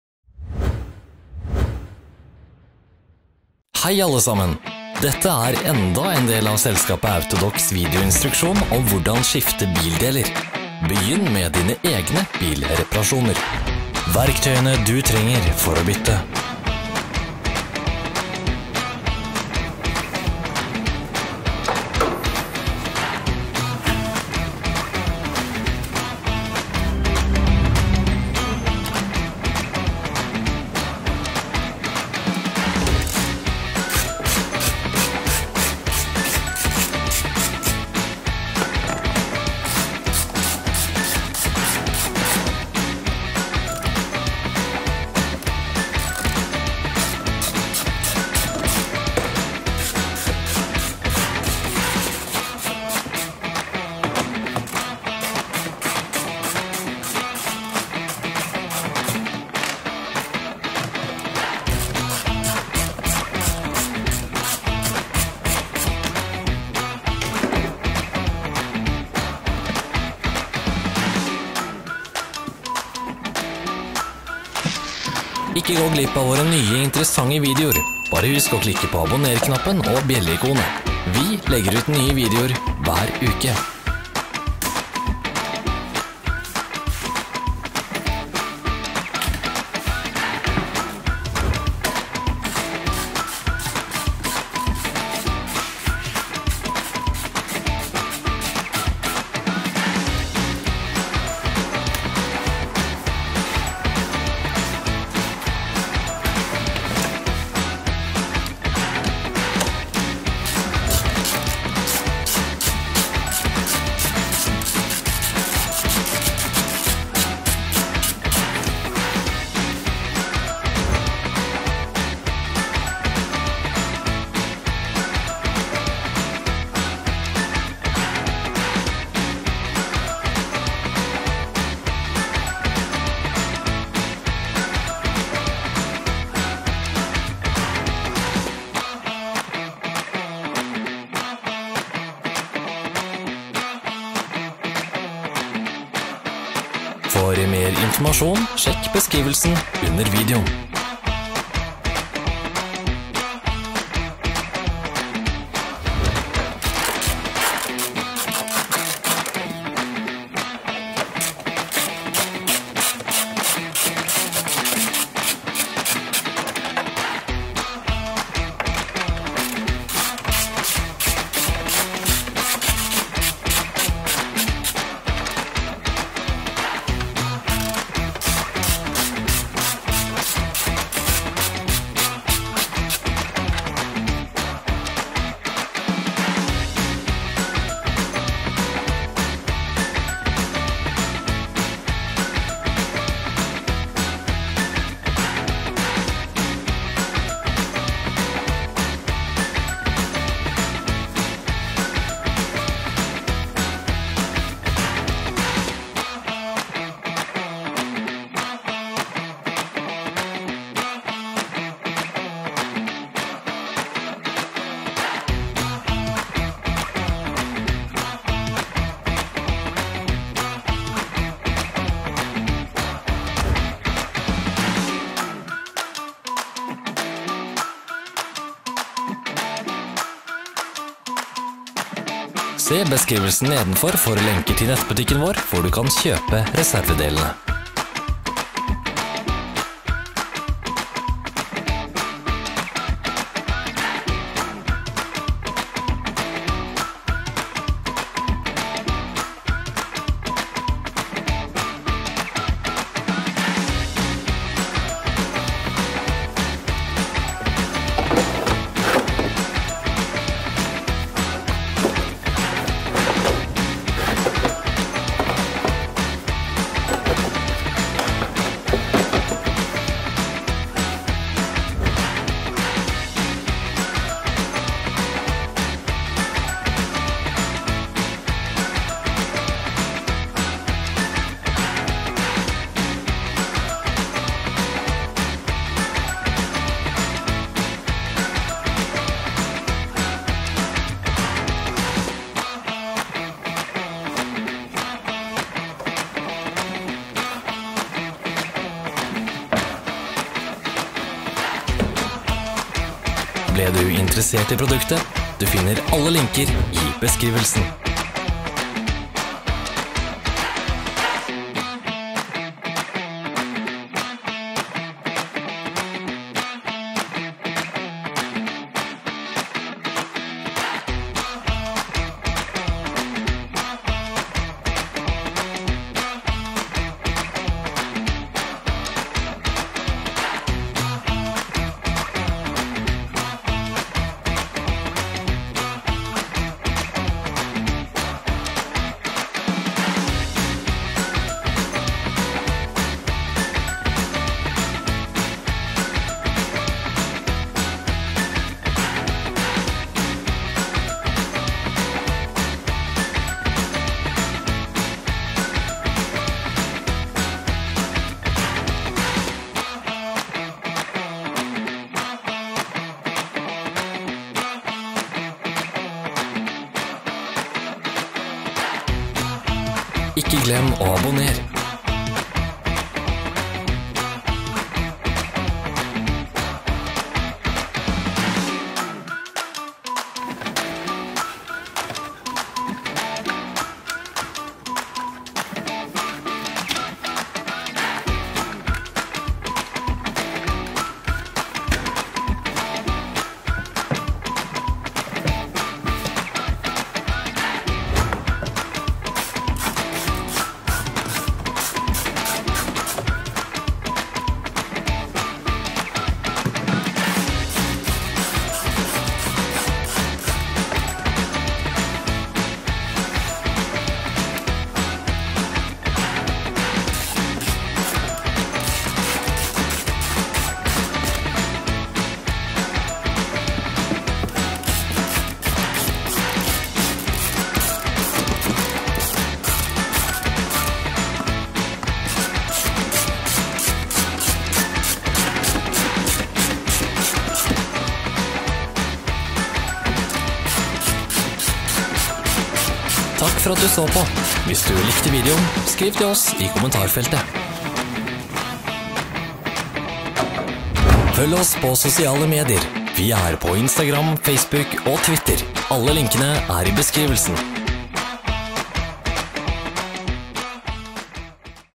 Hvordan bytte tennspole der på BMW 1-serie E82 [AUTODOC-VIDEOLEKSJONER]
Hvordan bytte tennspole på BMW 1 (E82) 3 Coupé 2007–2013 [AUTODOC-VIDEOLEKSJONER]

Tennspole til kjøretøyet BMW 1 Convertible (E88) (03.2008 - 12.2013) / BMW 1 Hatchback (E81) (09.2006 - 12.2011) / BMW 1 Hatchback (E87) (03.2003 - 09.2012) / BMW 3 Saloon (E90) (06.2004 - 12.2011) / BMW 3 Coupe (E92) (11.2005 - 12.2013) bør som regel erstattes i én og samme prosedyre. Det er imidlertid noen små forskjeller, på grunn av spesifikke egenskaper ved kjøretøyets design.

Alle video- og PDF-veiledninger om utskifting av deler i BMW (E82): last ned og se på AUTODOC CLUB

0:00 — Hvordan skifte tennspolen på BMW E82
1:08 — Fjerne kupèfilterhusdekselet
3:02 — Fjern motordekselet.
3:45 — Fjern også tennspolen fra hullet til tennpluggen i sylinderhodet.
4:01 — Installer også den nye tennspolen i hullet til tennpluggen i sylinderhodet.
4:23 — Monter motordekselet og fest det.
6:33 — Installer kupèfilterhusdekselet